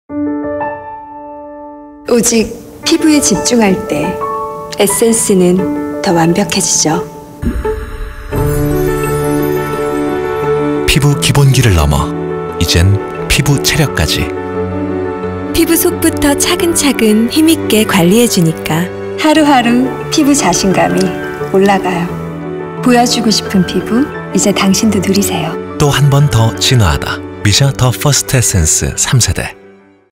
[미샤] 더 퍼스트 트리트먼트 에센스 3세대
또 한번 진화된 투명함과 촉촉함
미샤 NEW 타임레볼루션 더 퍼스트 트리트먼트 에센스 인텐시브 모이스트
2세 보다 더욱 좋아진 효능을 직접 확인해보세요!